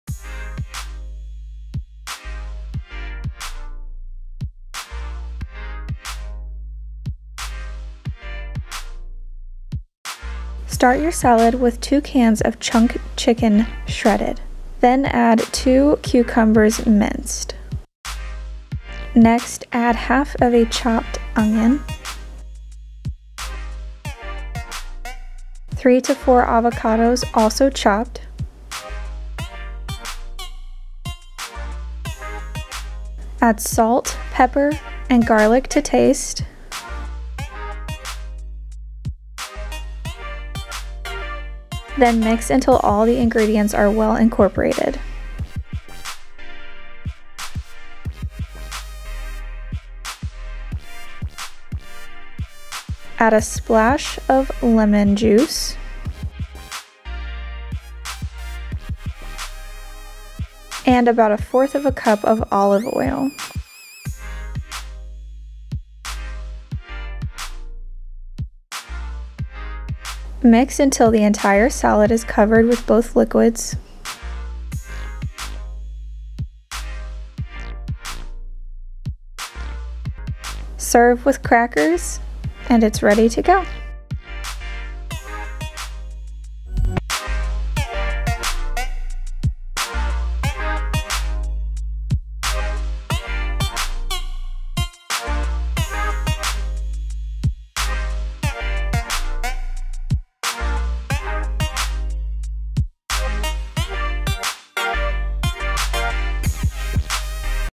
Avocado Cucumber Salad With Chicken / Best Easy Healthy Summer Snack
If you're looking for an healthy and easy snack to whip up in minutes then look no further! This snack is simple but packs a powerful tasty punch - not to mention it can feed a crowd!

Pyrex Glass Mixing Bowls